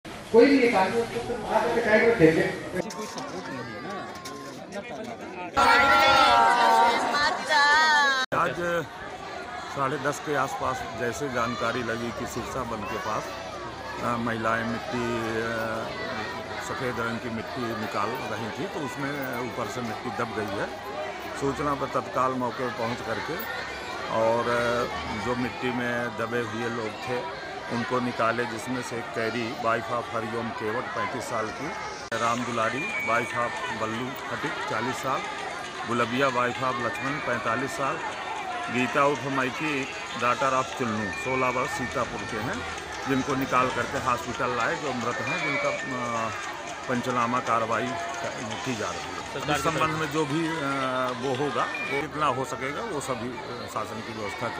MP में चूने की खदान धंसने से UP के 3 महिला मजदूरों समेत 4 की मौत
चित्रकूट : मध्य प्रदेश क्षेत्र में चूने की खदान धंस जाने से चार लोगों की दबकर मौत हो गई. इसमें एक किशोरी और तीन महिलाएं शामिल हैं. ये सभी सफेद मिट्टी की खुदाई कर रही थी. इसी दौरान यह हादसा हो गया. मृतकों में दो महिलाएं और एक किशोरी चित्रकूट के उत्तर प्रदेश क्षेत्र की रहने वाली थीं.